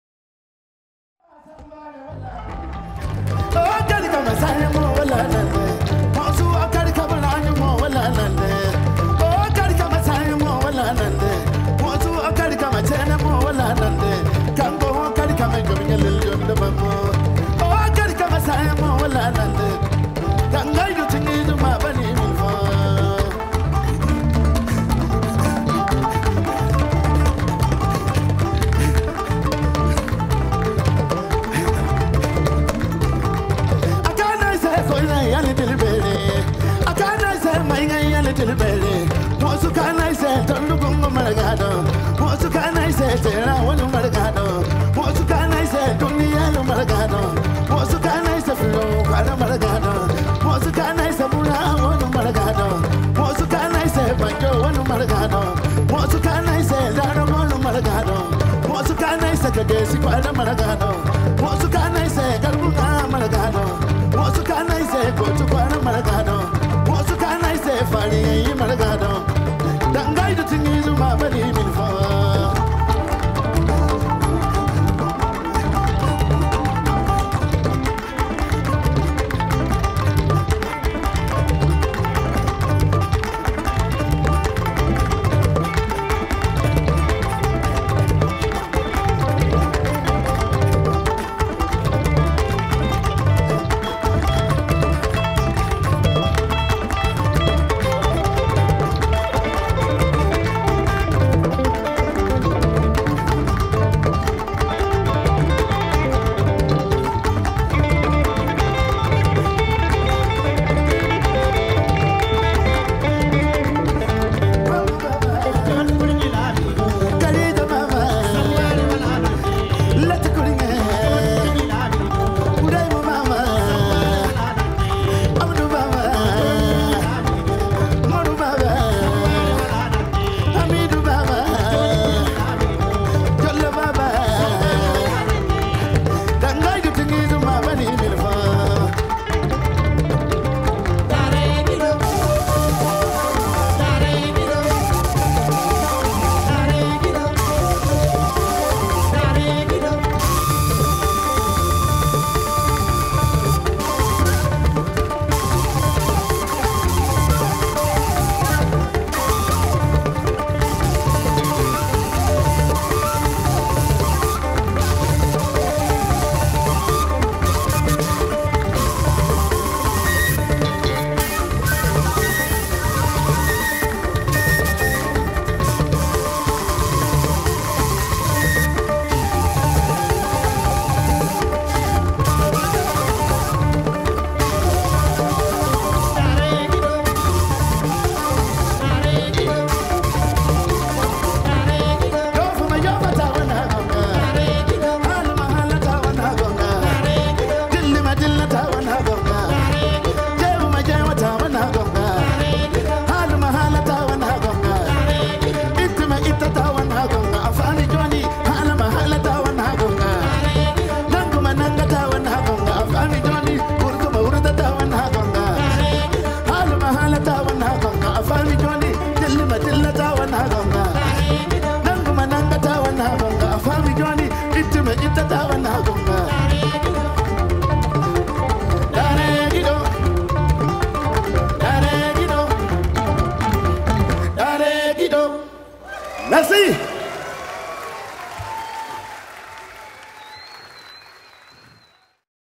Mamar Kassey Live in Womex Budapest 2015
Mamar Kassey
Live at Womex 2015, in Budapest

Yacouba Moumouni : chant, flûte, kora
Abdourahamane Albarka Bombou : guitare
Ousmane Abdou : Kalangou
Seyni Halidou : Molo
Abdoulkarim Mamadou N'Diaye : basse
Boubacar Souleymane : calebasse, chant
Hadiza Aloumaazou : danse, chant
The sound of Mamar Kassey is unique and firmly modern, a sound that knows where it comes from and where it is going. The obsessive and generous groove is a tribute to « god tempo. »
This willingness to defend the cultural richness of West Africa, to build bridges and foster tolerance between all social components of this vast region : Fulani, Tamaschek, Haoussa, Songhai, signify that Mamar Kassey, without giving into artifice and cliché, delivers a message of peace and tolerance. An uncompromising music, rich and exhilarating. »

crédit :  LAFONTAINE Jacques-Yves